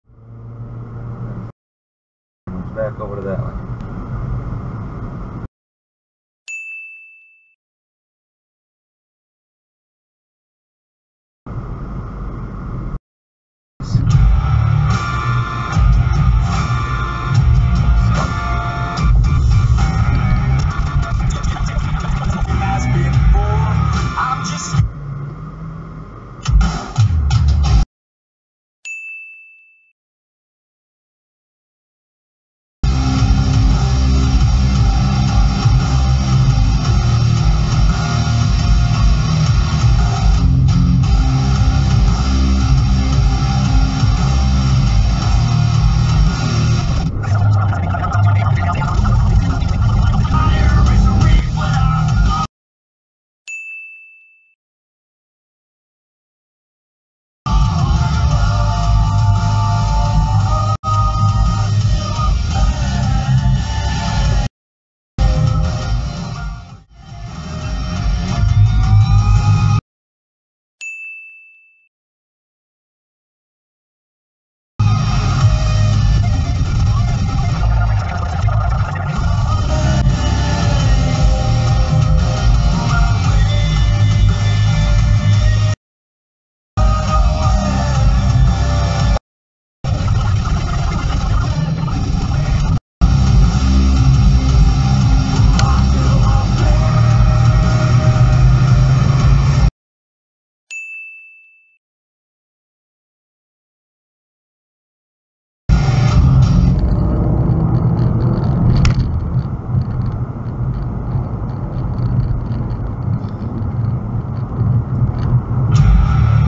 1/28/21 - 2 Clips
1/28/21 - Worcester Mass.

First clip - We start off with a moron in a Mercedes making a left from a straight only lane. Then fast forward a sec and some clown in a Nissan starts rolling before the light turns green and then proceeds to run the next red light.
Second clip - D-bag in a Toyota merges behind me cutting off a guy in a Nissan (not the same guy from earlier). Then the Nissan driver jumps the gore getting onto the highway and goes right to the left lane.